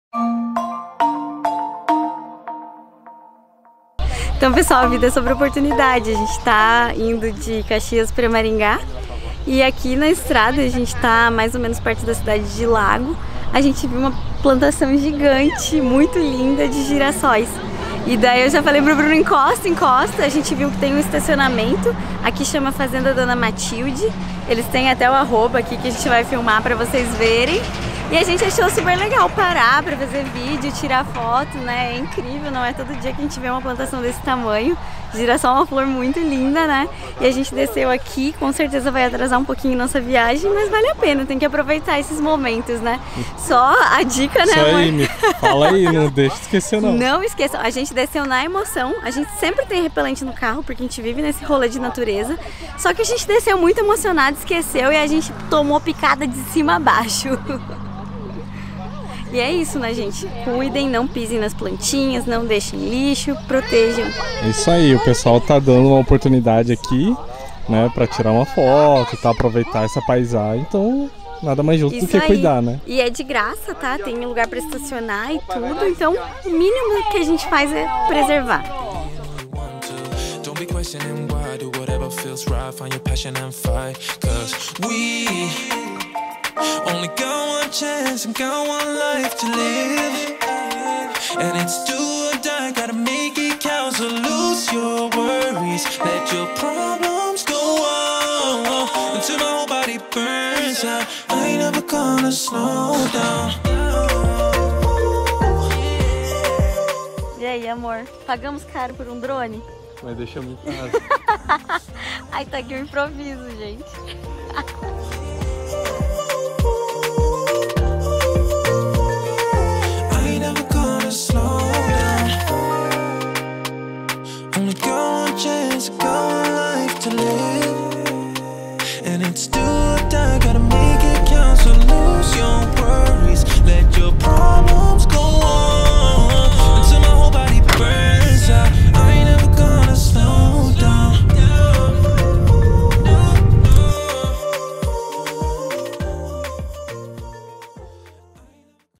VISITAMOS UM CAMPO DE GIRASSÓIS!
As vezes a estrada prepara algumas surpresas pra gente, e essa definitivamente foi uma delas. No meio do longo caminho entre Caxias do Sul e Maringá, achamos um oásis, um campo enorme de girassóis. Claro que paramos na hora e já descemos pra gravar tudo pra vocês e pra fazer fotos bem lindas. Essa maravilha fica no município de Lago, perto de Palmeira, no Paraná, mais especificamente na @fazenda_donamathilde, e a visitção é gratuita (pelo menos em junho de 2022). Espero que gostem!